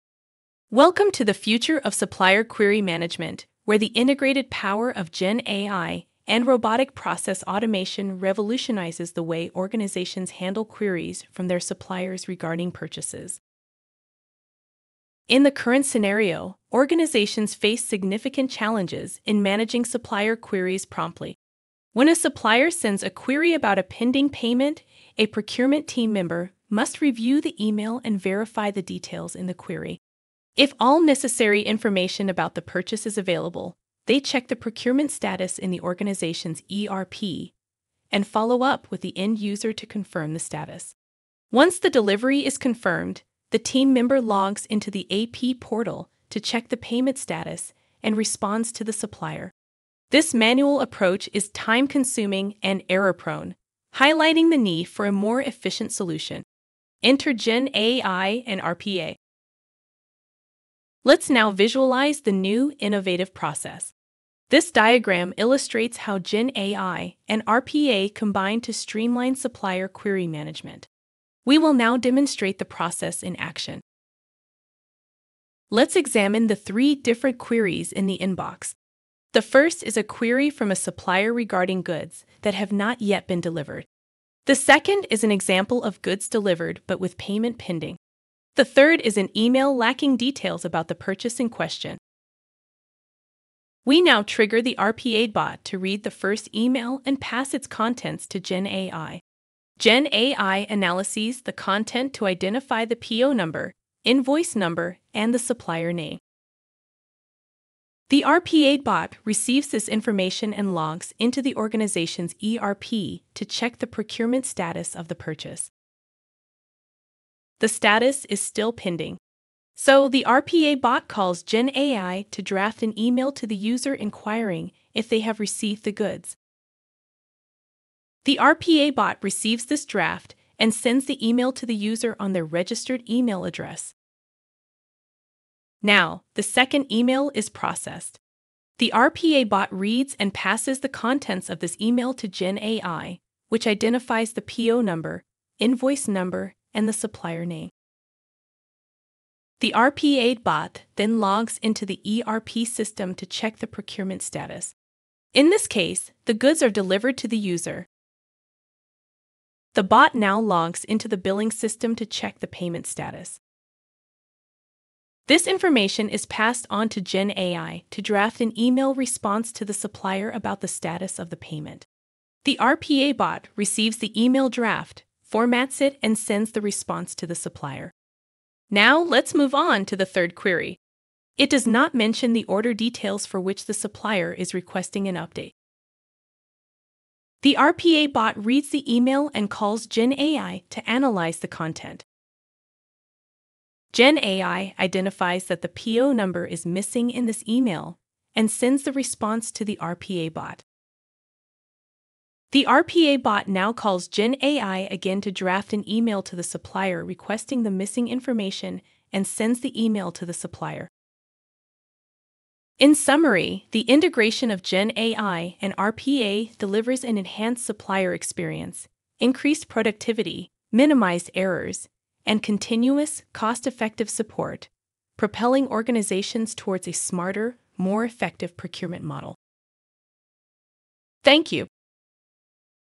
Integration of Intelligent Automation and Generative AI for Supplier Query Management
This demo shows the future of supplier query management, where the integrated power of Intelligent Automation and Generative AI revolutionizes the way organizations handle queries from their suppliers regarding purchases.